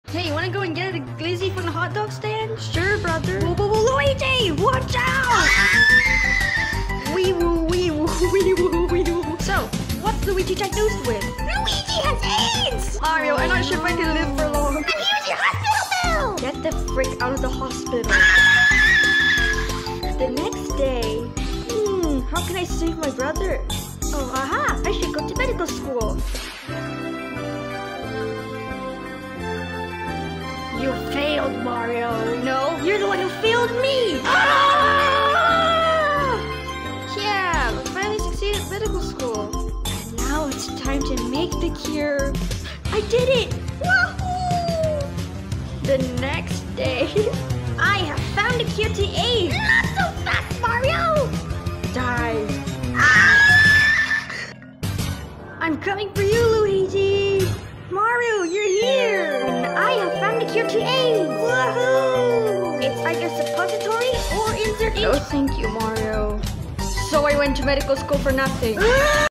that one dr mario ai brainrot meme but i voiced over it while i'm going insane
i was too lazy to do the italian accent oops